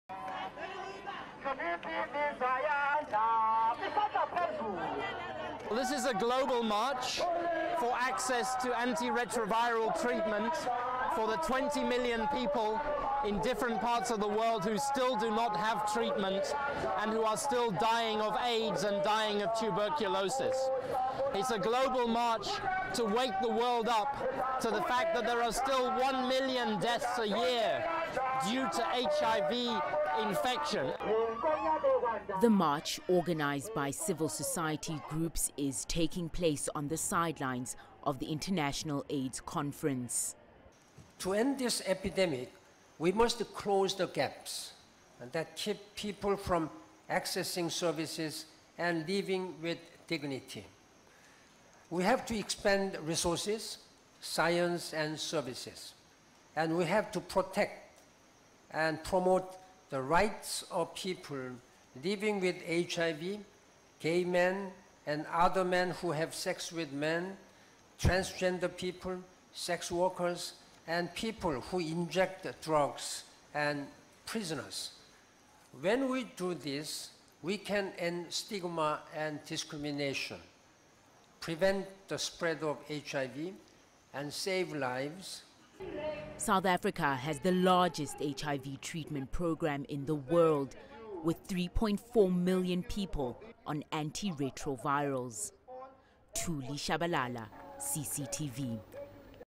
South Africans call for wider access to HIV treatment
As the International Aids Conference in Durban goes on, thousands have been marching  to call for better access to HIV treatment.